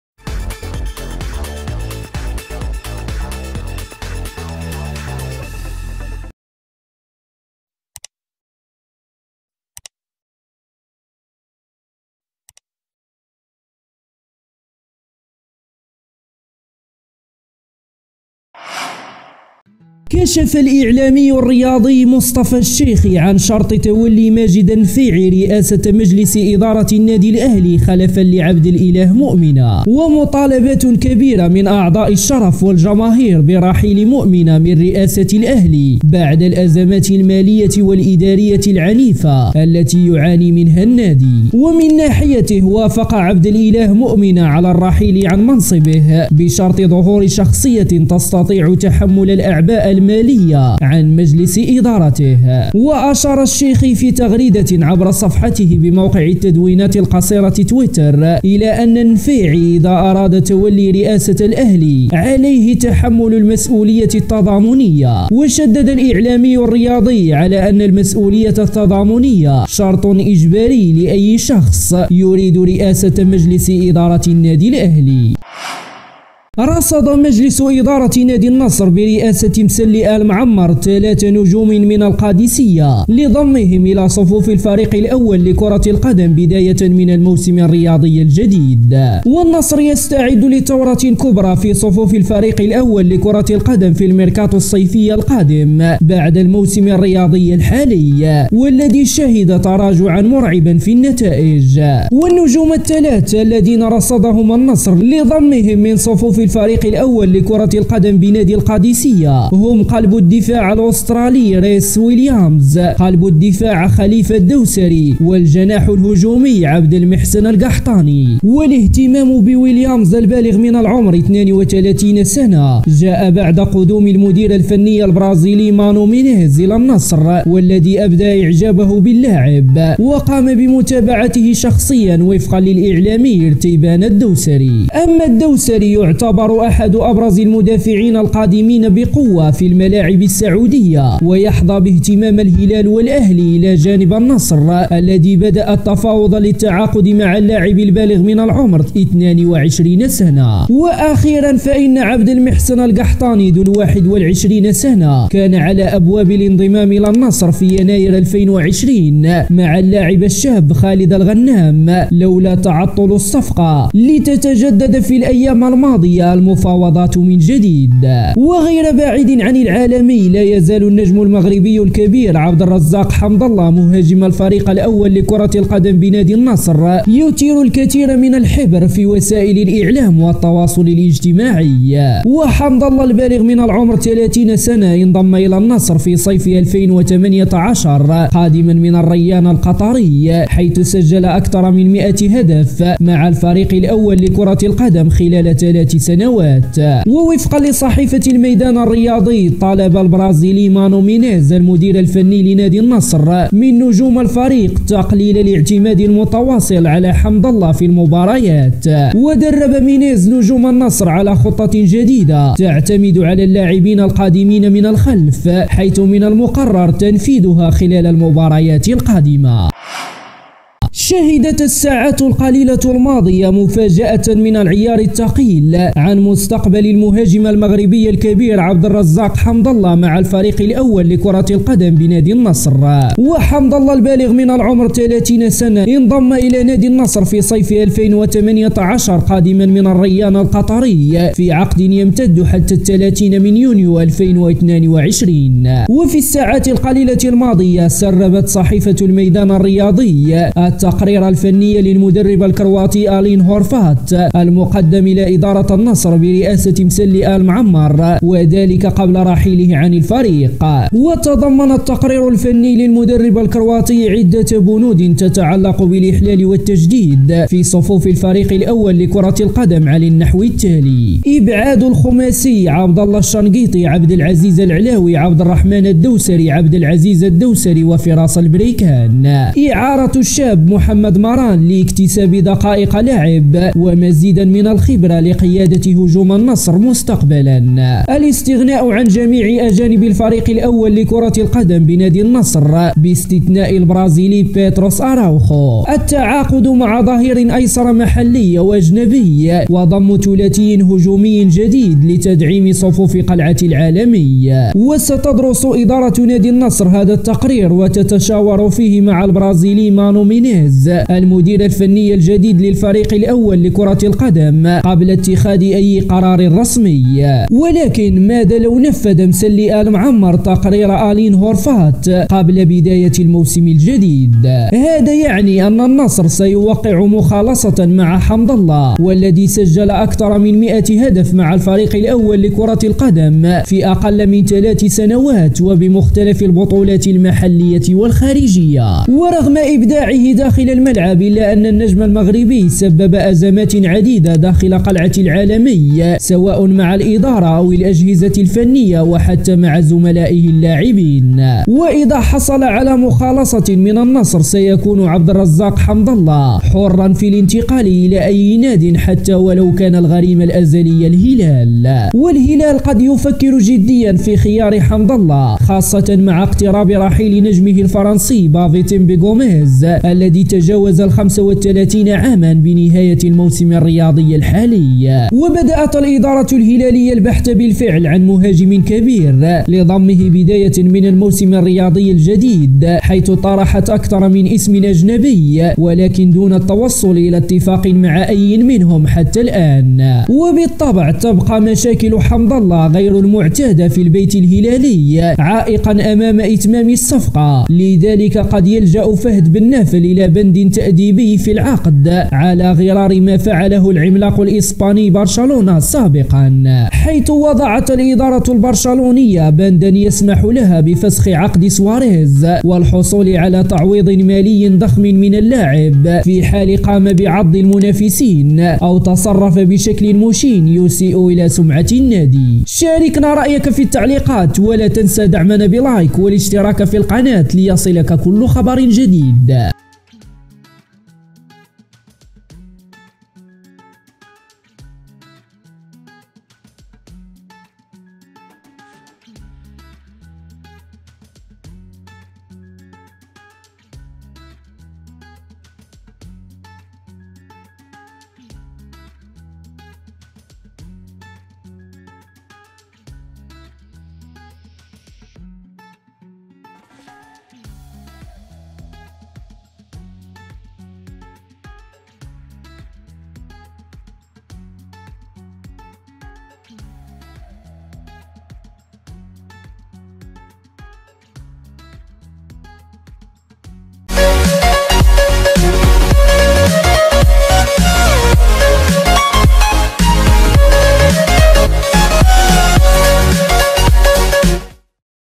بن نافل يوافق على تعاقد الهلال مع حمد الله|مسلي آل معمر النصر يتسوق من القادسية|رئيس الأهلي الجديد
▬▬ஜ۩سوكر_نيوز ۩ஜ▬▬
2- أرجو منك تفعيل الجرس 🔔 لتكون اول من يعلم بالاخبار
3- ولا تنسى اللاااايك 👍 سيفيدني كثيراآ ❤
▬▬ஜ۩سوكر_نيوز ۩ஜ▬▬